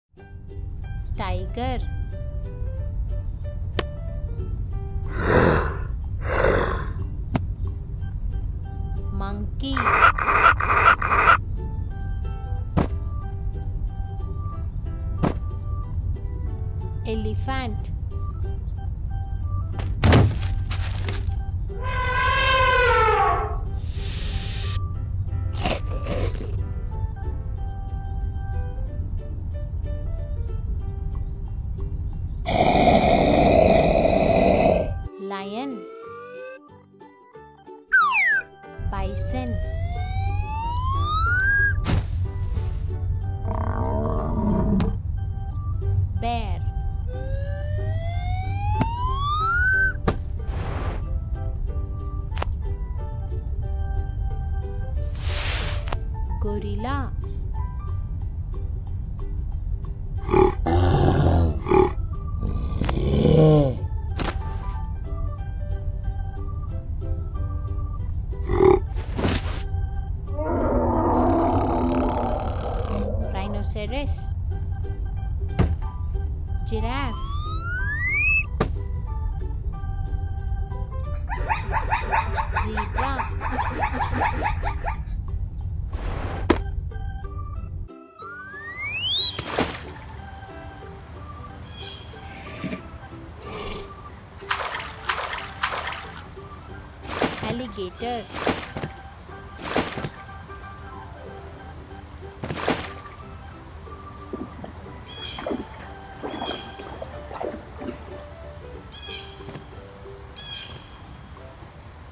Wild Animals in Forest | Short Cartoon Film for Kids
Wild Animals in Forest | Cartoon Film for Kids

Also called as Animals in Wild, is a funny short film about Wild Animals in Forest.
Tiger, Monkey, Elephant, Lion, Bear, Gorilla, Rhinoceros, Giraffe, Zebra and Alligators are part of the video.